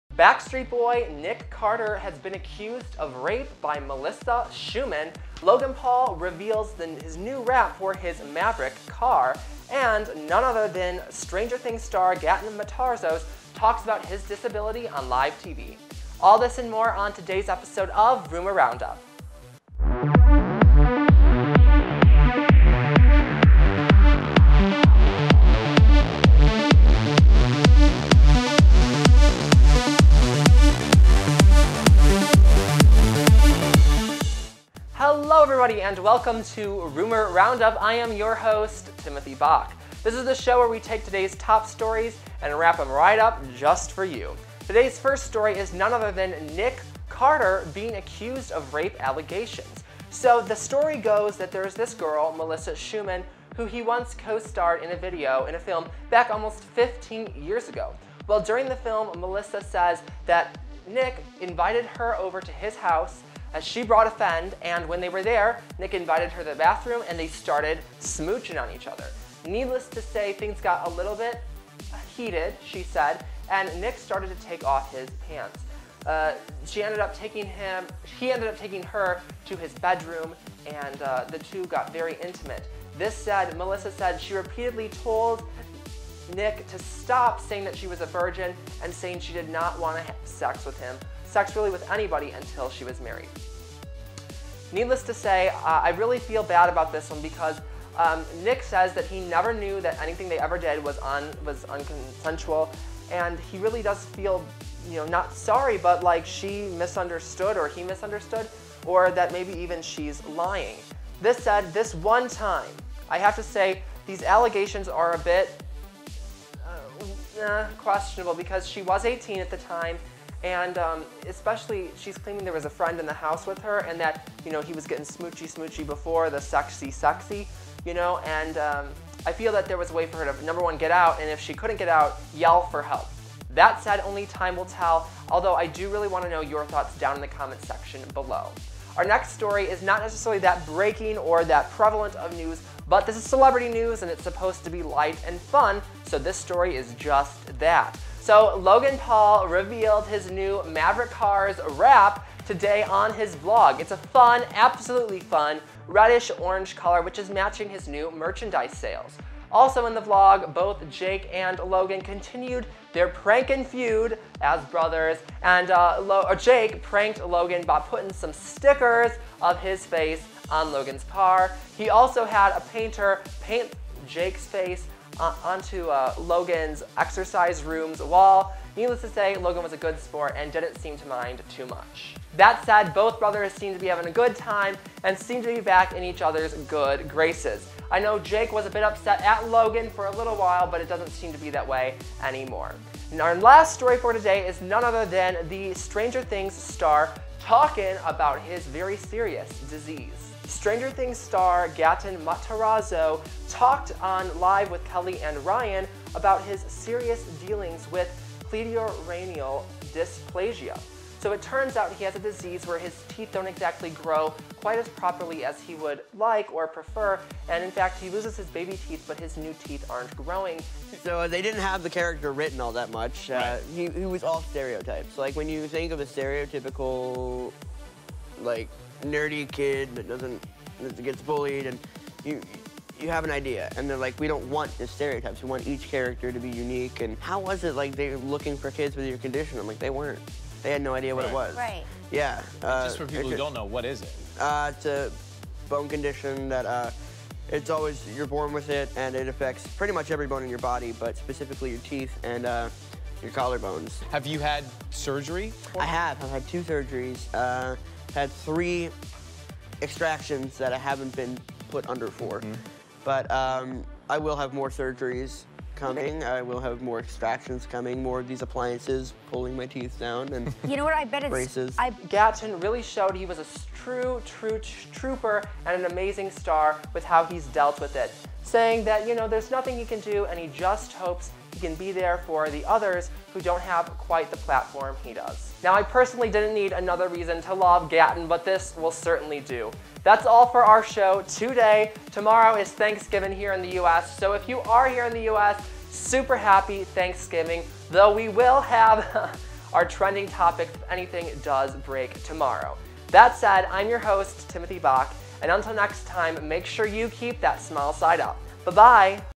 Stranger Things Star Gaten Matarazzo On Cleidocranial Dysplasia, Nick Carter Rape Allegations
Yet another big name celebrity has been accused of sexual misconduct and these most recent accusations are reaching the music industry.

Everyone knows and loves the Backstreet Boys so earlier today, when rape allegations surfaced of band member Nick Carter, fans were extremely surprised.

Earlier today, accusation surfaced, claiming that Nick Carter raped fellow musicial Melissa Schuman back when they were both acting in a film together 15 years ago. Schuman claims that she and a friend were invited to Carter’s home and she was invited to the bathroom. The two started kissing and eventually Carter started to unbutton and remove his pants. Schuman is quoted as stating, “He threw me on the bed and climbed on top of me. Again, I told him that I was a virgin and I didn’t want to have sex. I told him that I was saving myself for my future husband. I said it over and over again. He whispered in my ear as to entice me, ‘I could be your husband”.

Creepy enough as that is, Schuman claims the situation only escalated from there.

She continued to depict the story, “He was relentless, refusing to take my no’s for an answer. He was heavy, too heavy to get out from under him. Then I felt it, he put something inside of me. I asked him what it was and he whispered in my ear once more, ‘It’s all me baby”. She claims that this attack caused her to not pursue a career in music and lead her to quitting the industry. This said, carter claims that he has no recollection of any non consensual sexual activities. In a statement released by the former Backstreet boy, Carter denied any knowledge of their relations being non consensual saying, “I am shocked and saddened by Ms. Schuman’s accusations. Melissa never expressed to me while we were together or at any time since that anything we did was not consensual. We went on to record a song and perform together, and I was always respectful and supportive of Melissa both personally and professionally.”

I’m going to be honest, I personally am not going to jump right to the alleged victims side on this one. For me, the story has a couple of holes, such as why Melissa didn’t leave the house immediately after Carter allegedly started to take off his clothes or at the least she could have called out for her friend. For me, it’s sounding a bit extortionary.

Y’all, Logan Paul just revealed the new wrap color for his smoking hot Maverick Car!

I know it’s not the biggest news of all time, but celeb news is supposed to be fun and this story is just that.

Today in his vlog, Logan Paul revealed the color of his car’s new wrap. Logan again partnered with his friends at West Coast Customs to pimp out yet another one of his vehicles. This time Logan wrapped his Dodge Challenger to match his newest Maverick Merch release. The car now sports a fun orangy red color which is sure to turn heads on the street.

That said, it wouldn’t be a Paul brother reveal without a special surprise, or should I say prank!

Prior to Logan’s arrival, Jake made it a point to plan a special surprise for Logan. Jake printed out window stickers of his face on an old ‘Dick and Jane’ picture and placed them on Logan's car. Jake also got Logan pack by having an artist paint Jake’s face on the wall of Logan’s new gym room. Needless to say, Logan was a good sport and didn’t seem to mind either prank.

Today on Live with Kelly and Michael, Stanger Things Star Gaten Matarazzo discussed what life is like living with Cleidocranial Dysplasia.

I’m pretty sure we were already extremely impressed with Gaten Matarazzo’s amazingly sweet spirit and acting ability, but today he gave us yet another reason why we love him!

Gaten appeared as a guest of Live with Kelly and Ryan early Wednesday morning and her told the hosts what life was like living with Cleidocranial Dysplasia. The star spoke about the seriousness of the disease and explained the many different surgeries and therapies he has had to endure. Video

Needless to say, I think this just gives us yet another reason why we all love Gaton.

I want to know why you guys love Gaton!

KEEP YOUR SMILE SIDE UP!